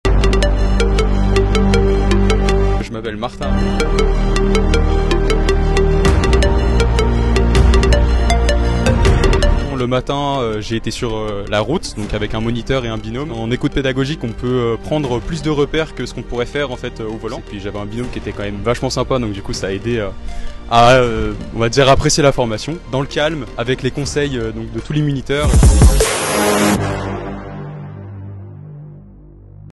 Ce qu'il vous attend cette semaine... Mystère ! - Témoignage
Cette semaine un peu de suspens... Martin a réalisé son stage de conduite chez Marietton et nous livre son ressenti !